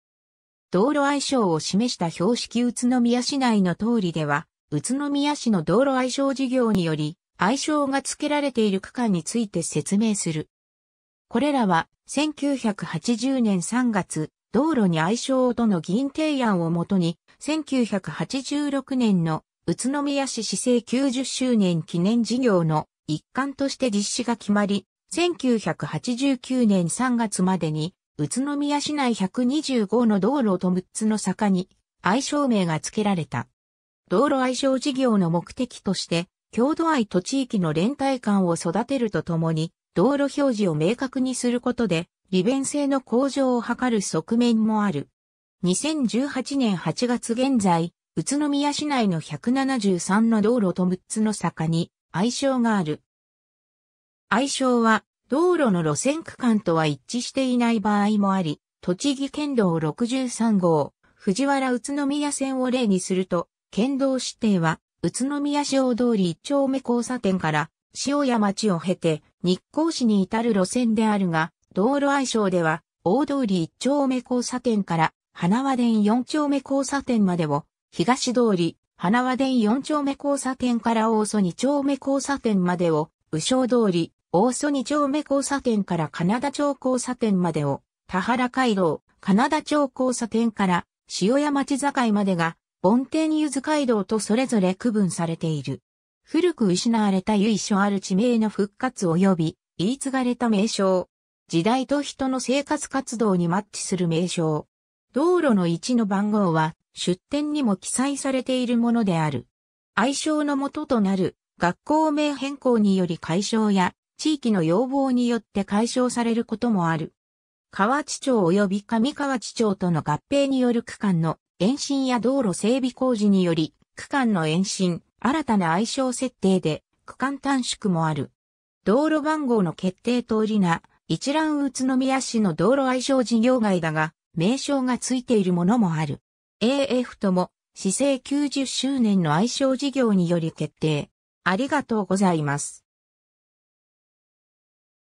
宇都宮市内の通り
道路愛称を示した標識 （競輪場通り） 宇都宮市内の通り（うつのみやしないのとおり）では宇都宮市の道路愛称事業により愛称がつけられている区間について説明する。
 これらは、1980年（昭和55年）3月「道路に愛称を」との議員提案をもとに、1986年（昭和61年）の宇都宮市市制90周年記念事業の一環として実施が決まり、1989年（平成元年）3月までに宇都宮市内125の道路と6つの坂に愛称名がつけられた。
道路愛称事業の目的として、郷土愛と地域の連帯感を育てると共に道路表示を明確にすることで利便性の向上を計る側面もある。
 2018年（平成30年）8月現在、宇都宮市内の173の道路と6つの坂に愛称がある。
 愛称は、道路の路線区間とは一致していない場合もあり、栃木県道63号藤原宇都宮線を例にすると、県道指定は宇都宮市大通り1丁目交差点から塩谷町を経て日光市に至る路線であるが、道路愛称では、大通り1丁目交差点から塙田4丁目交差点までを「東（あずま）通り」、塙田4丁目交差点から大曽2丁目交差点までを「宇商通り」、大曽2丁目交差点から金田町交差点までを「田原街道」、金田町交差点から塩谷町境までが「梵天ゆず街道」とそれぞれ区分されている。
 古く失われた由緒ある地名の復活および言い継がれた名称。
 時代と人の生活活動にマッチする名称。
 道路の1～の番号は、出典にも記載されているものである。
 愛称の基となる学校名変更により改称や、地域の要望によって改称されることもある。
 河内町および上河内町との合併による区間の延伸や道路整備工事により区間の延伸、新たな愛称設定で区間短縮もある。
 道路番号の決定 通り名一覧 宇都宮市の道路愛称事業外だが、名称がついているものもある。
 A～Fとも、市制90周年の愛称事業により決定